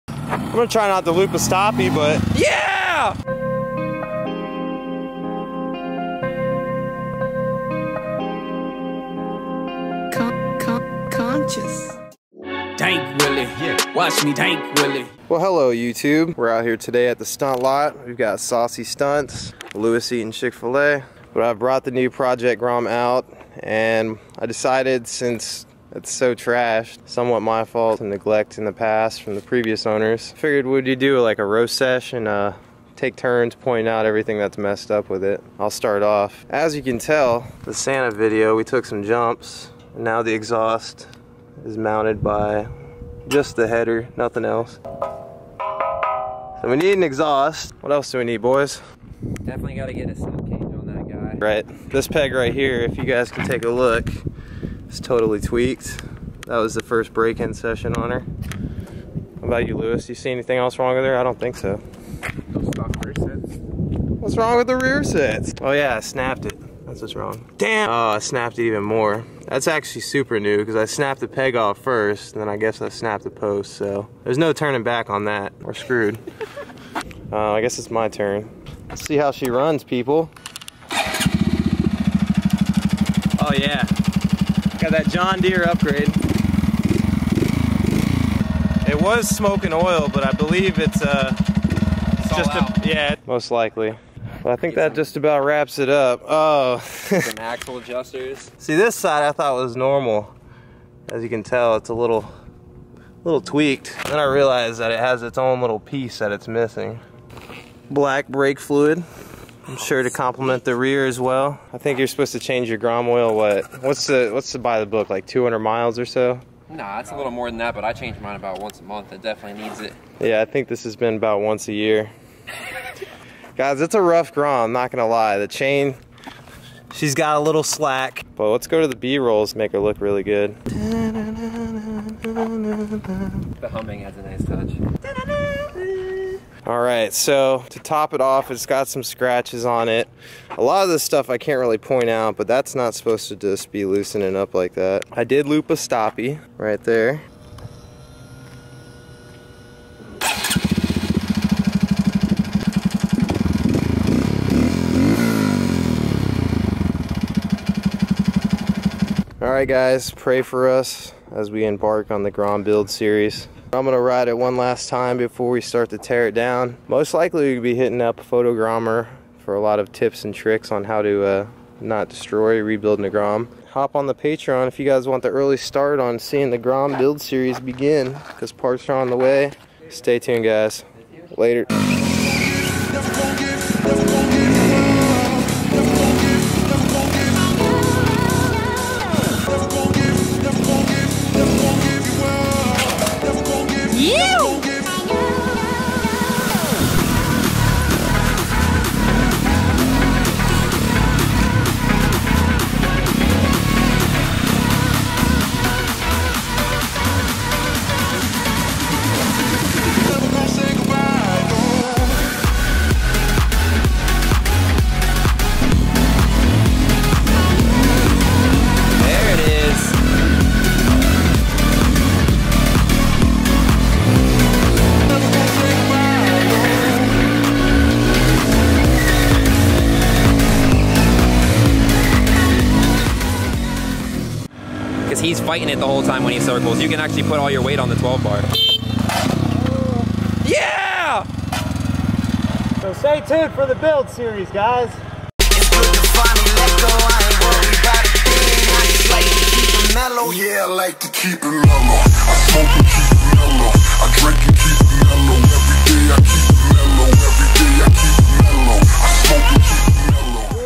How's The Grom Doing?
So today I met up with @saucystunts and Luis eating chik-fil-a to go over all the things wrong with my new grom. A roast session felt necessary to do so, The grom has been put on the back burner lately with several other projects going on but then again as basic of parts already on it she's already holding strong through my riding abuse lol

BIKE: 2015 Kawasaki Ninja ZX6R (636)

GEAR:
Helmet - Icon Airmada Nikova 2
Jacket - Icon Team Merc Jacket (Yellow/Black)
Gloves - AlpineStar Brawlers

CAMERA:
- GoPro Hero 4 Silver (1080P 60fps superview)
- Sam Ash GoPro Mic lavalier

@Moramoto / Moramoto.com